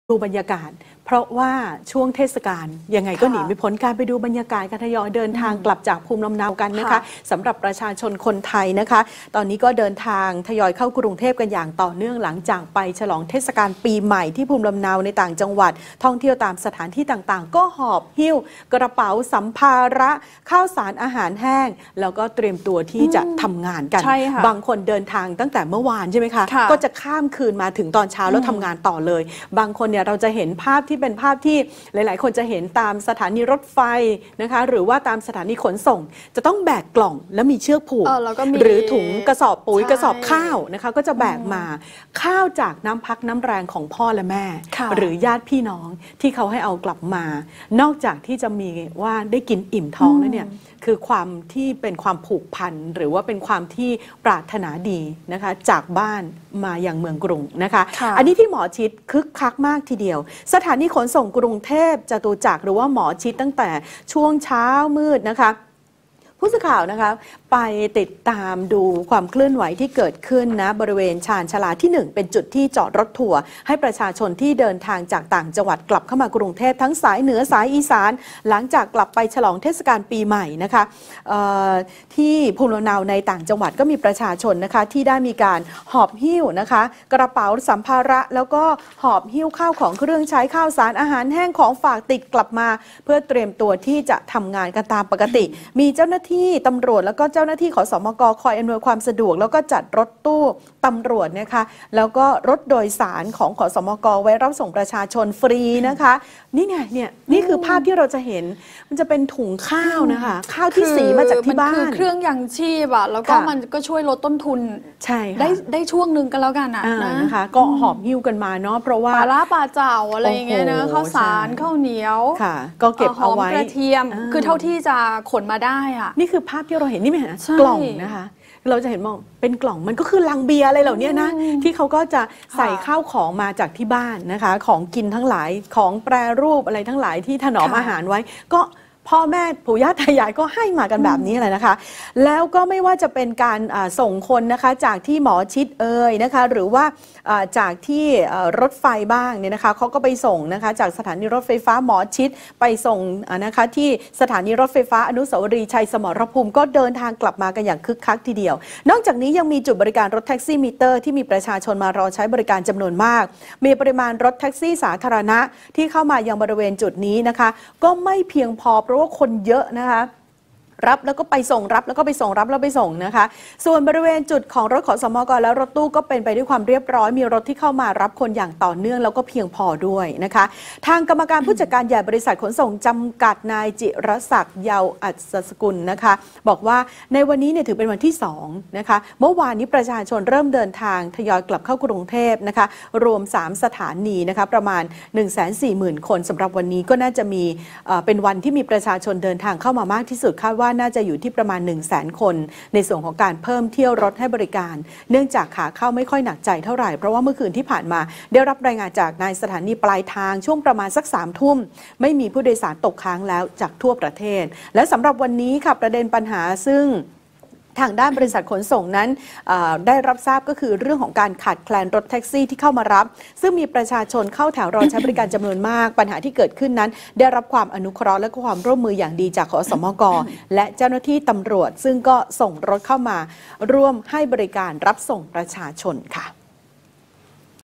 หมอชิตคึกคัก ปชช.เดินทางกลับหลังหยุดปีใหม่
02/01/2020 หมอชิตคึกคัก ปชช.เดินทางกลับหลังหยุดปีใหม่